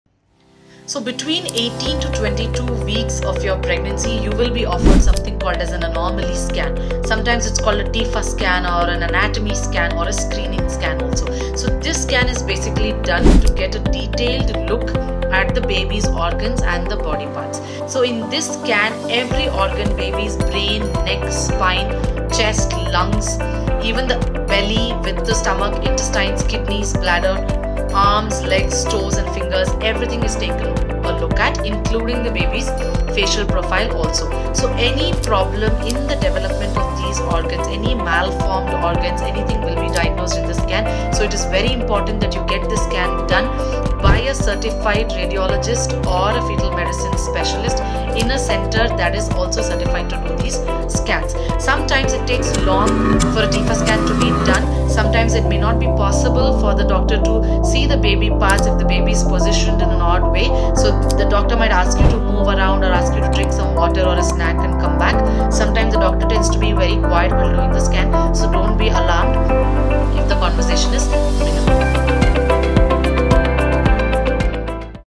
Understanding Pregnancy TIFFA Scan: Importance of Anomaly Screening | Dr Manasa Badveli
TIFFA scan means “Targeted Imaging For Fetal Anomalies”

Some malformations are not lethal and can be corrected through surgery after baby’s birth.

Some malformations are major & inoperable & can cause baby’s death inside the uterus or immediately after birth. Such pregnancies may need to be terminated after the scan diagnosis.